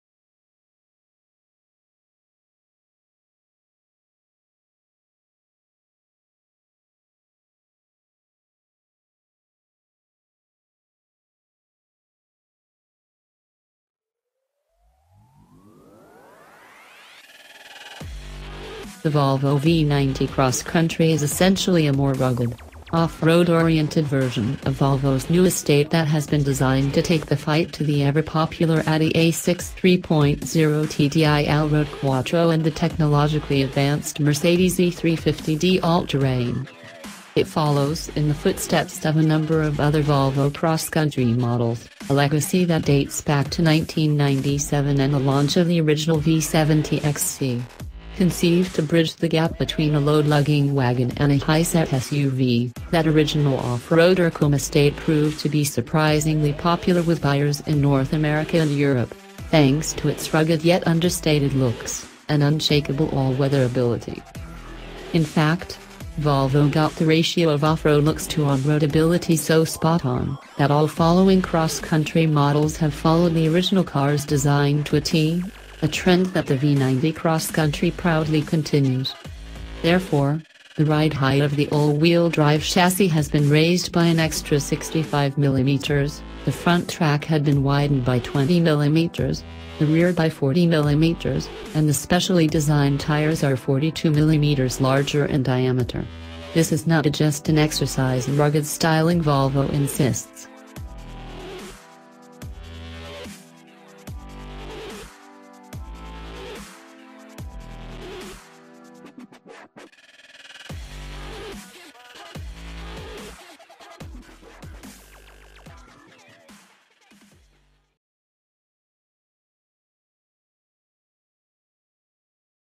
Hot News !!!! 2018 Volvo S90 Interior and Infotainment Overview
Hot News ! 2018 Volvo S90 Interior and Infotainment Overview - Volvo claims the S90 represents a greater threat to the German dominance of the executive market than any of its cars have before. And it undergoes this road test in current entry-level form: a £33,000, 187bhp, Momentum-spec diesel.

██████ GET DAILY UPDATES! NOW! ██████